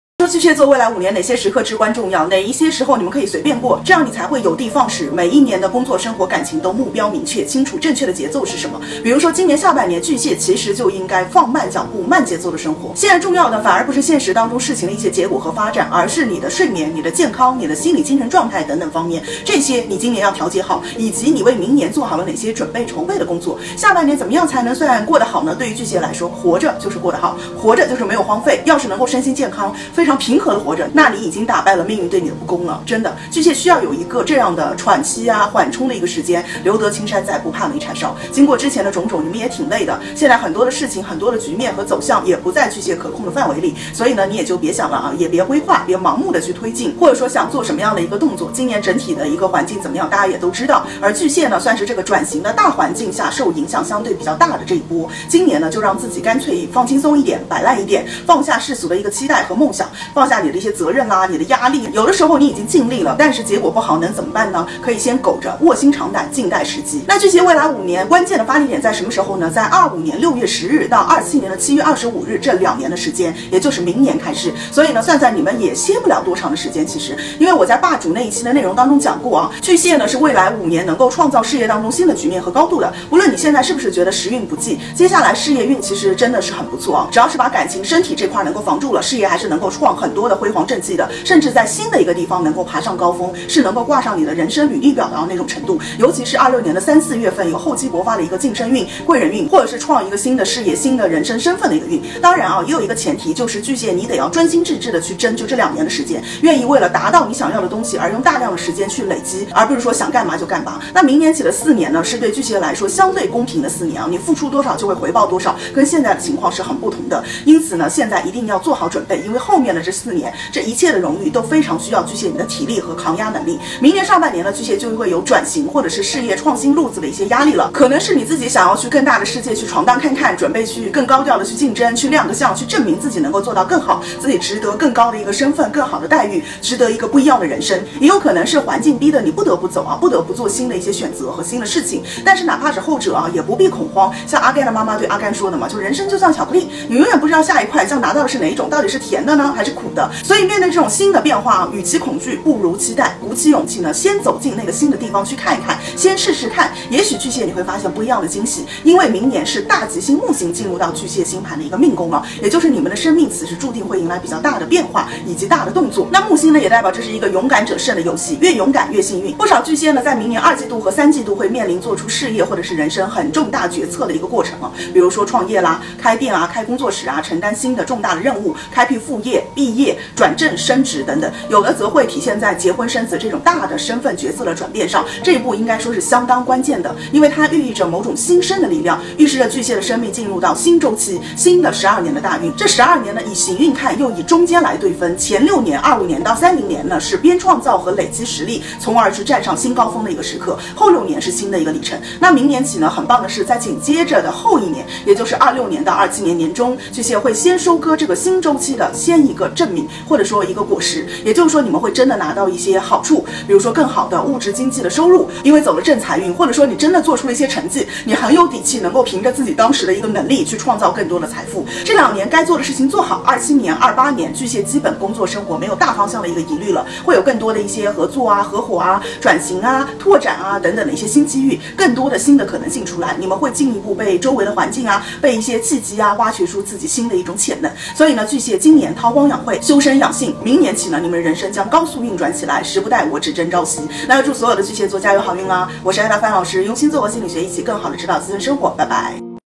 巨蟹座从24年到28年 未来五年哪年顶重要？ ！提前规划，轻松成功！十二星座
这里是星座心理学Ada范老师
如果有对星座感兴趣的小可爱
星座运势解析，带你了解星座
持续更新哦~感谢订阅支持！